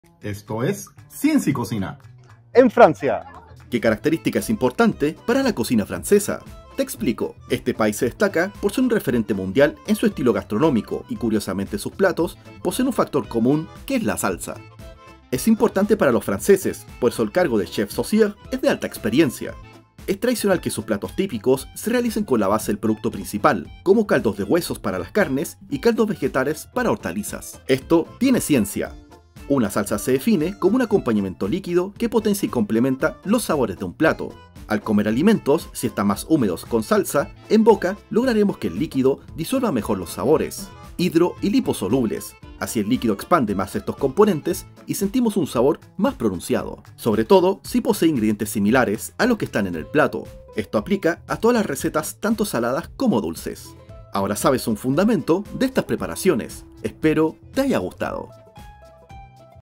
¿Que es lo más característico de la Cocina Francesa?
La cocina en Francia es reconocida a nivel mundial. Su gastronomía y técnicas están presentes en muchos países. Sobre todo en la educación del cocinero. Aquí te explico lo más relevante de la cocina francesa con la ayuda de la ciencia!